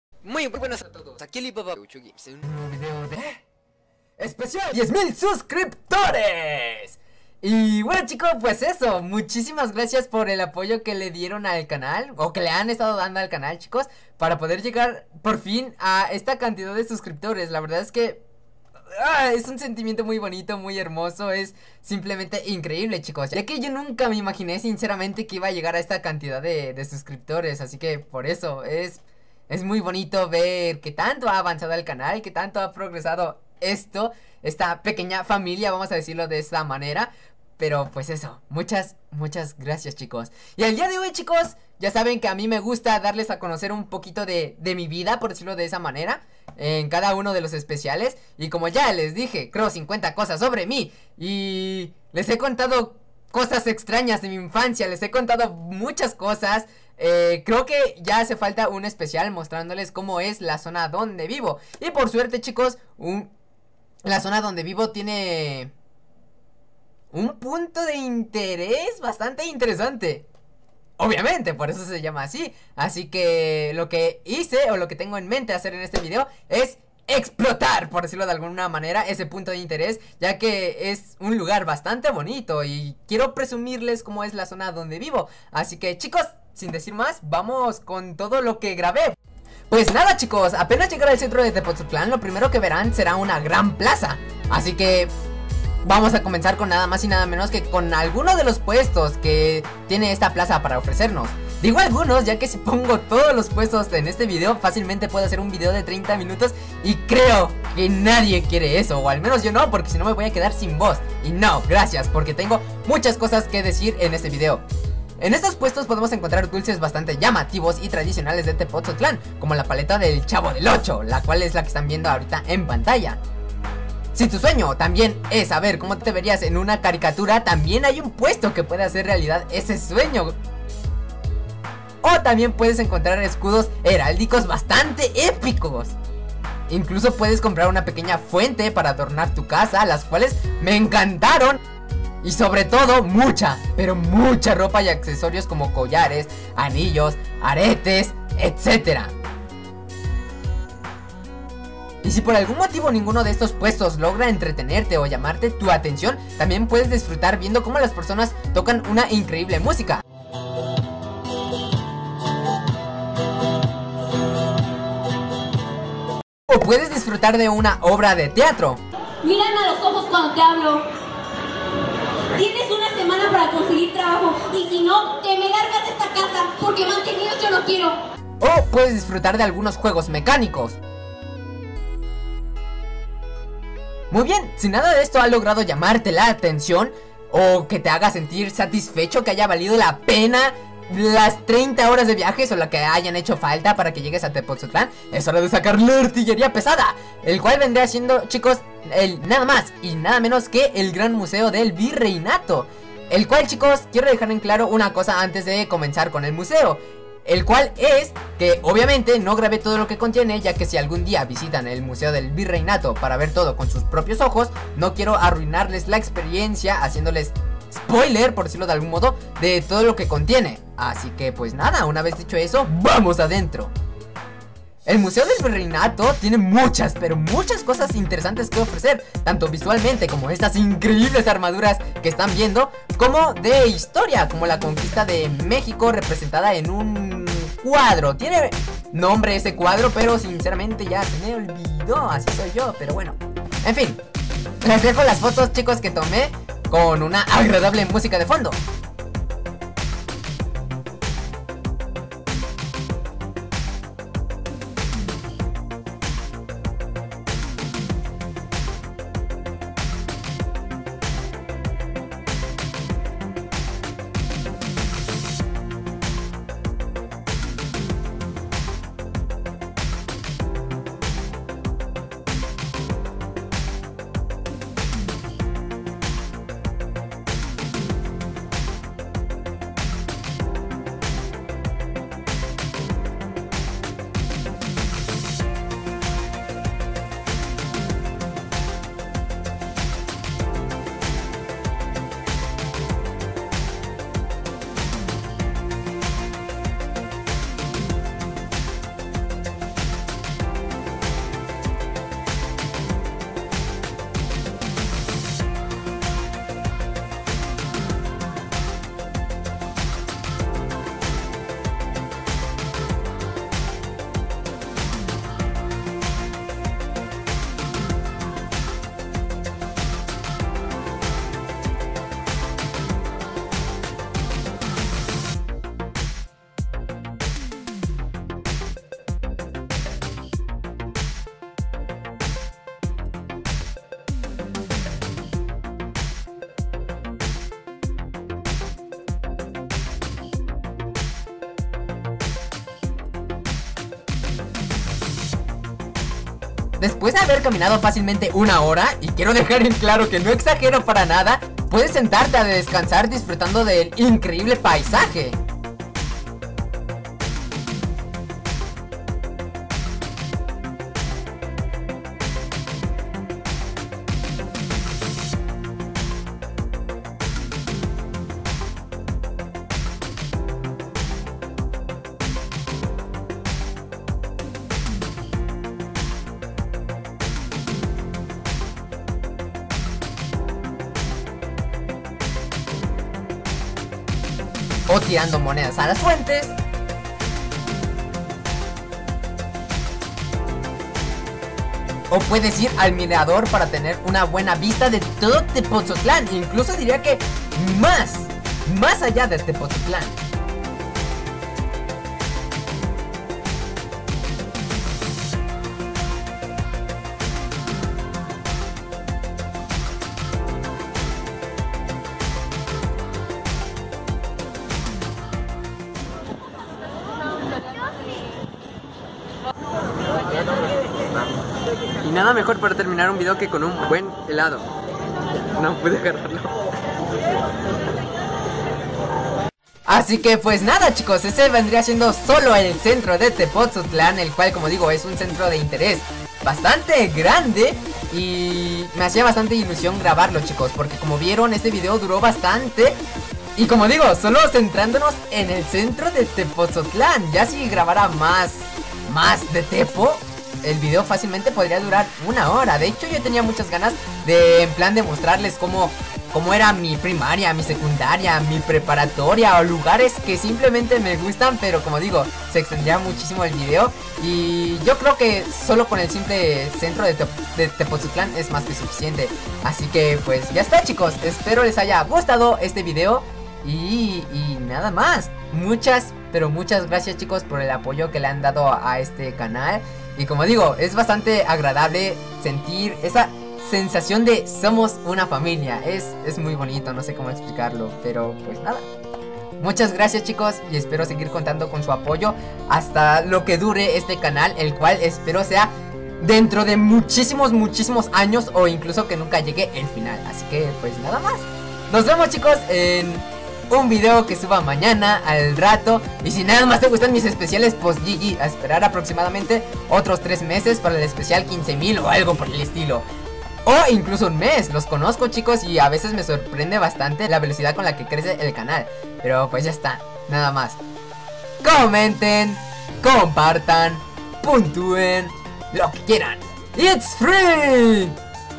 Especial 10000 Suscriptores! | Les presento mi municipio ~ Tepotzotlán | Alibabav8 Games
Muy buenas!:3
Espero les guste este Especial 10 mil suscriptores! :D
Chicos, enserio muchisimas gracias por todo el apoyo que le dan al canal, son los mejores :3
PD: disculpen mi pulso de maraquero n_n)'' estaba algo nervioso ya que nunca habia grabado un blog